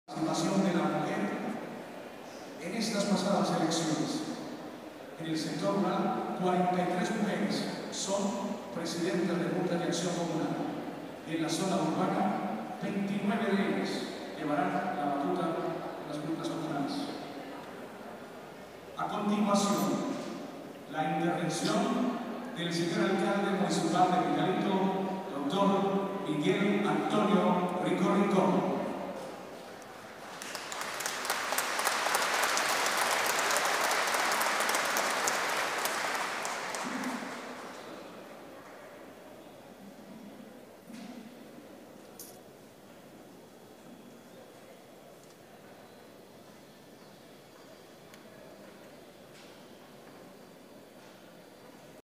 Dirigentes comunales asistieron a su acto de posesión en Pitalito
Ante  el  Gobernador del Huila; Carlos  Julio Gonzáles  Villa;  el alcalde del municipio  Miguel Antonio Rico Rincón,  concejales y gabinete   municipal.  Hoy   finalmente  se ha  cumplido el  acto de  posesión  de los dirigentes  comunales en el  Valle  de Laboyos.